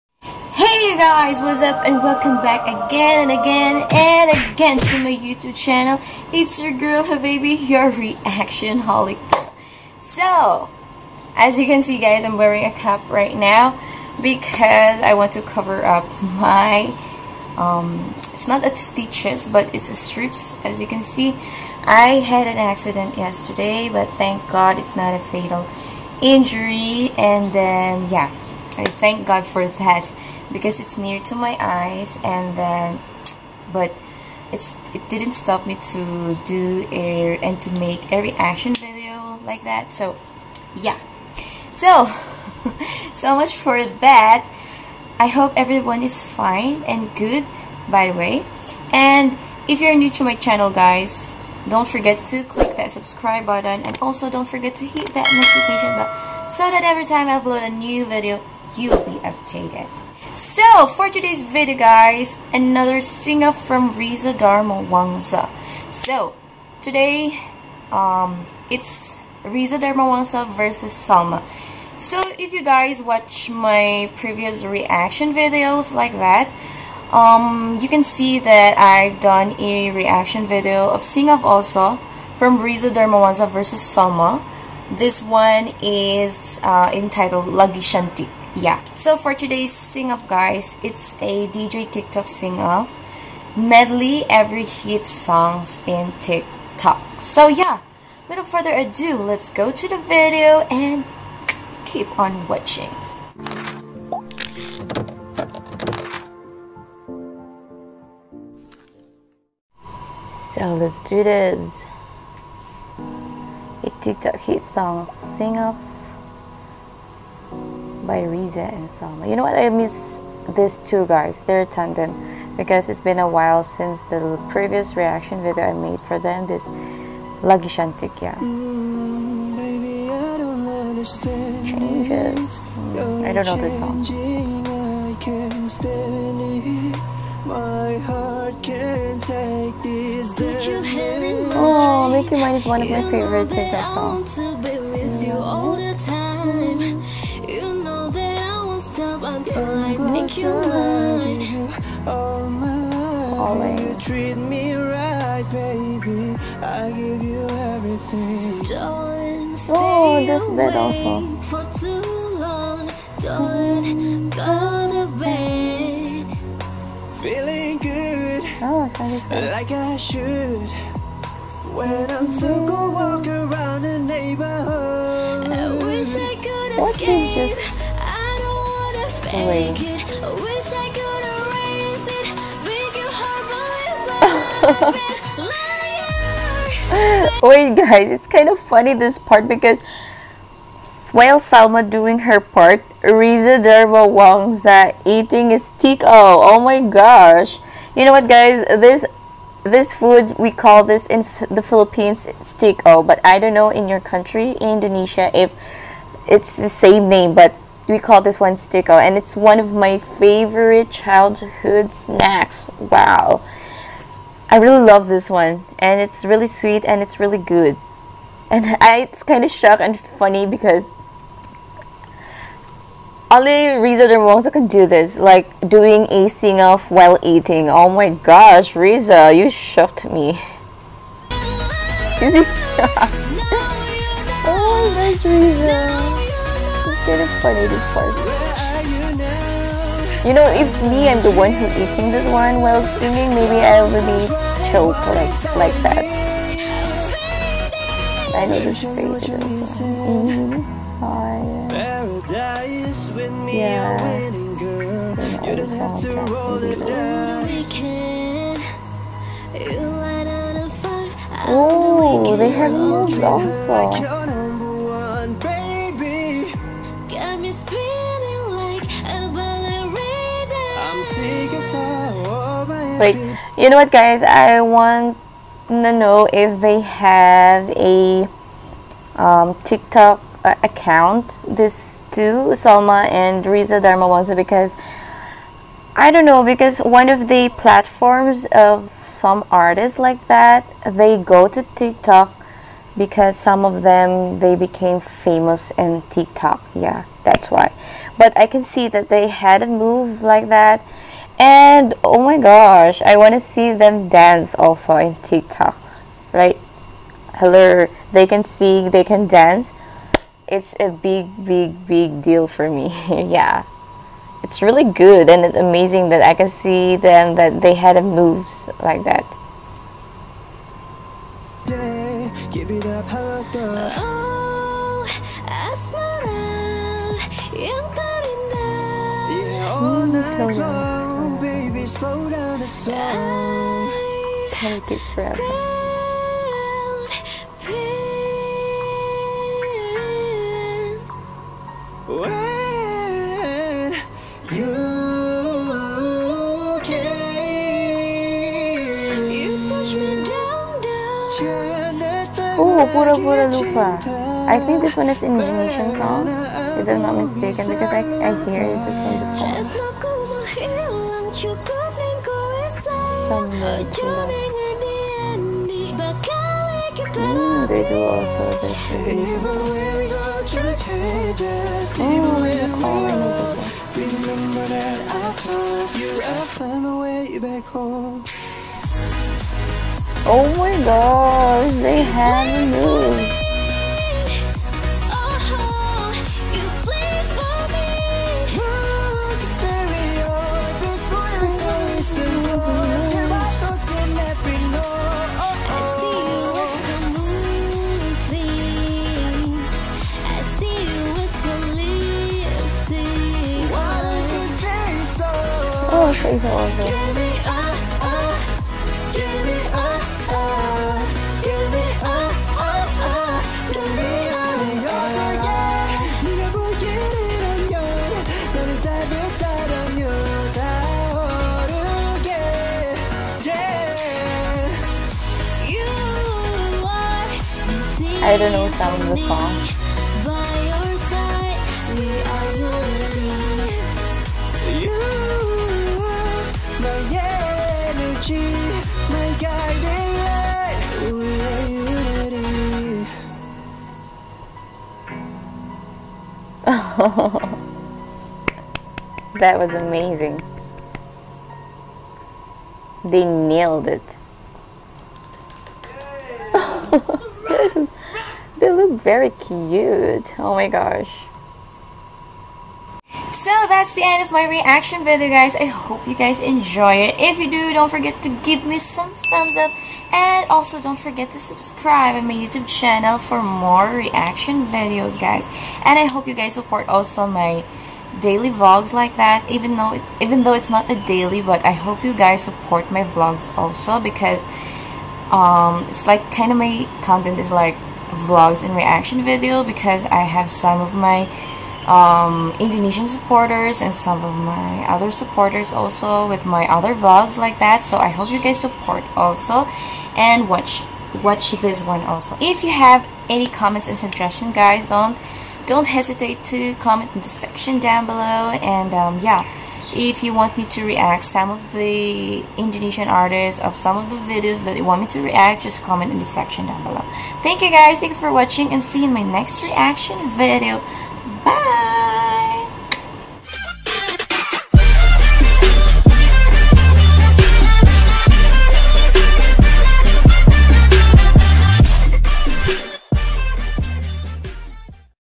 DJ TIKTOK SING-OFF (MEDLEY EVERY HIT SONGS ON TIKTOK) VS SALMA | REACTION VIDEO
Hello habibies another Reaction video from RezaDarmawangsa SING-OFF vs Salma.
They make a medley from the hits songs on tiktok. I hope you guys love it and support this one. Make this Blue 💙 if you love Reza Darmawangsa and Salma.

Mailing Address
(Just in case you want to send me something)

FLORYVIC MONTECILLO
Purok 2, Barangay Salvador, Sierra-Bullones, Bohol, Philippines, 6320